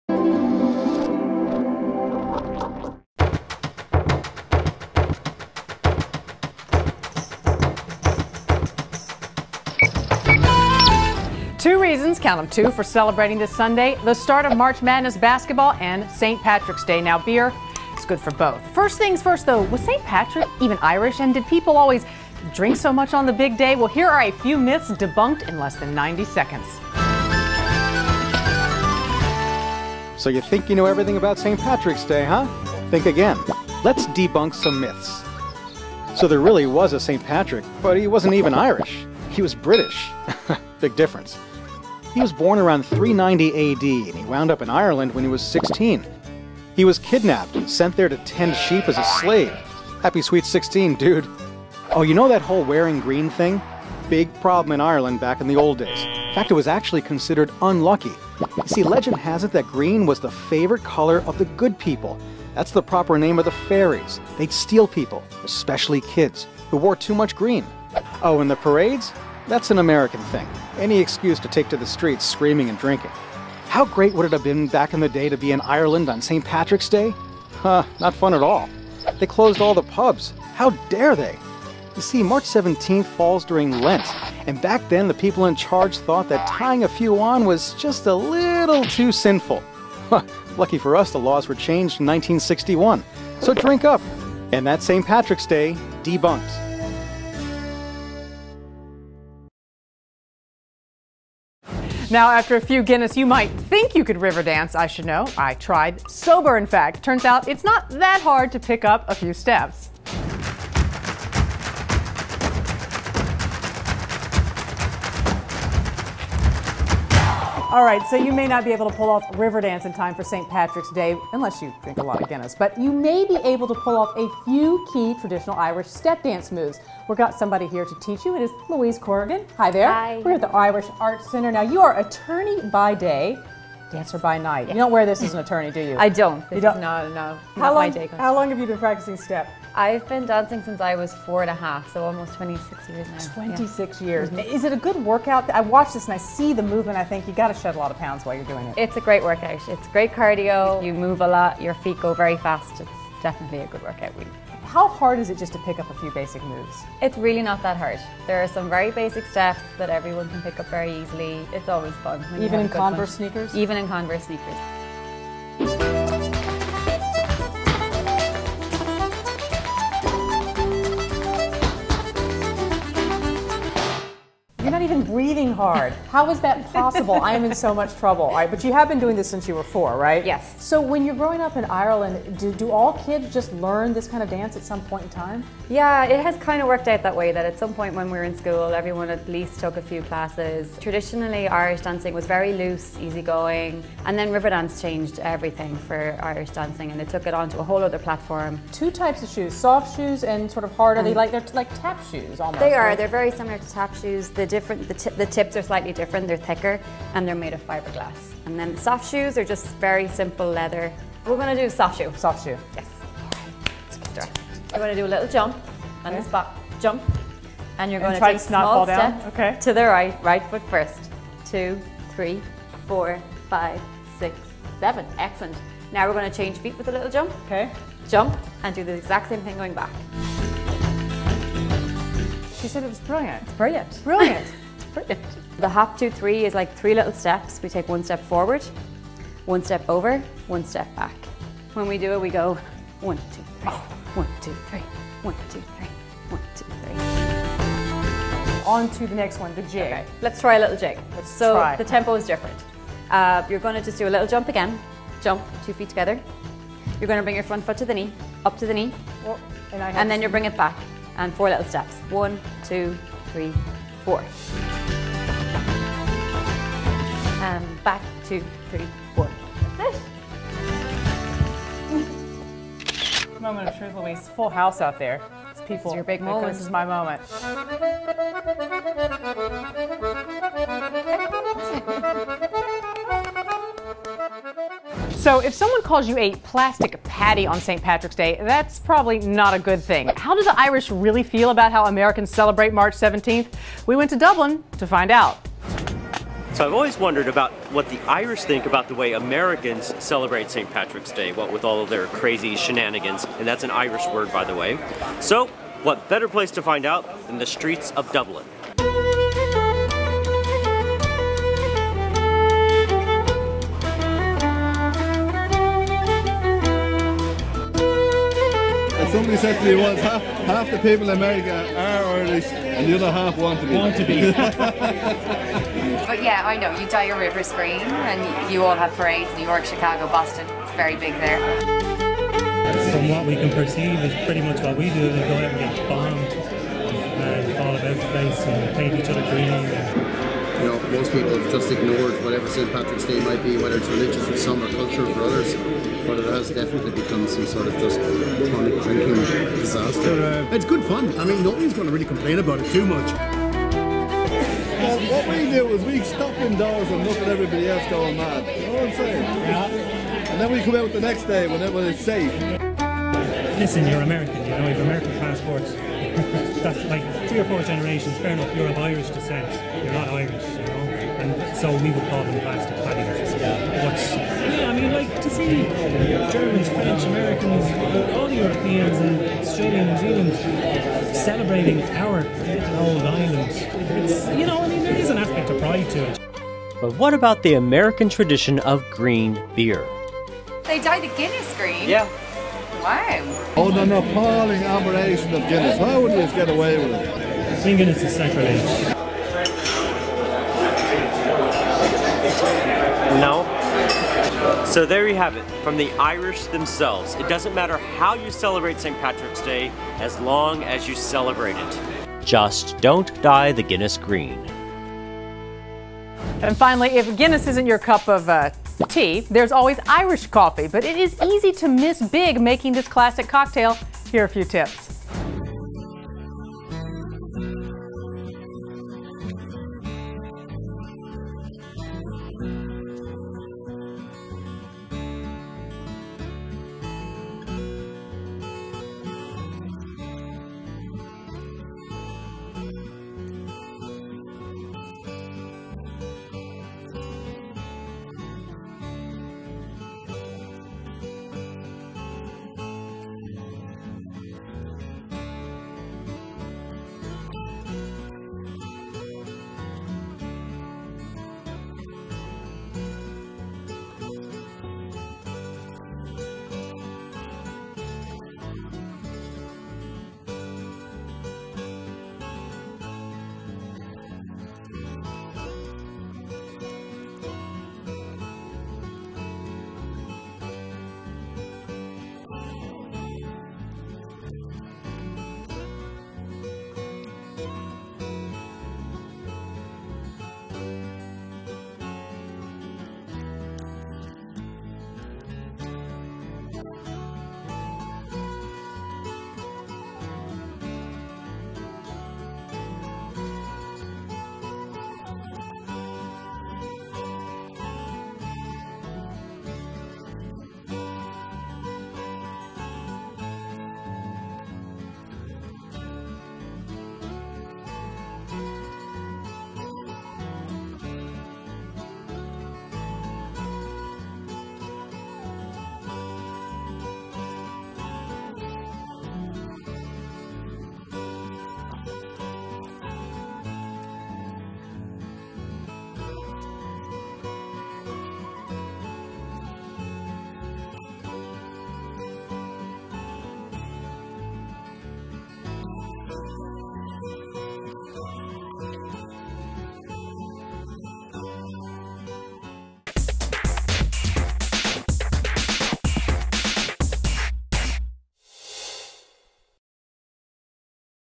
Irish Fever: Myths, Riverdance & Plastic Paddys
Was St. Patrick even Irish? Will drinking green Guinness make you a Plastic Paddy? And can you learn to Riverdance in Converse sneakers? WSJ Off Duty seeks answers.